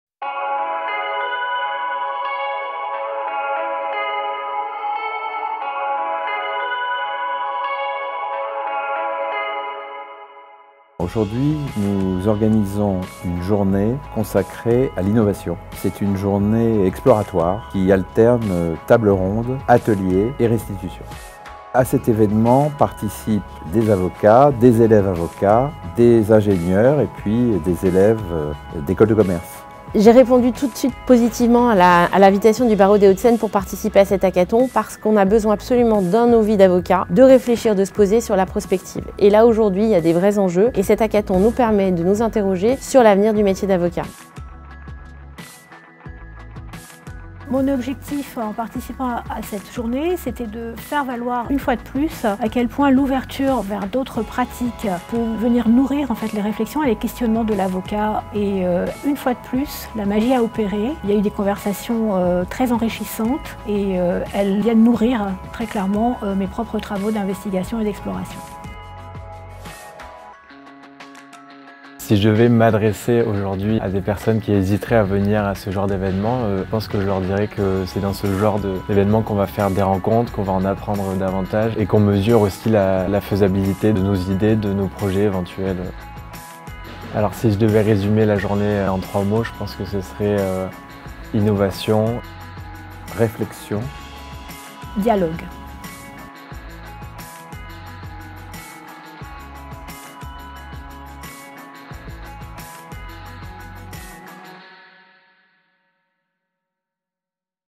Récits d'une journée d'exploration avec le Barreau des Hauts de Seine
Et de 3 !
Déjà 3 ans que nos équipes accompagnent l’incubateur AvocatsLab92 du Barreau des Hauts de Seine.
De l’inspiration, l'exploration à l’accompagnement de projets legaltech, découvrez leurs témoignages en images.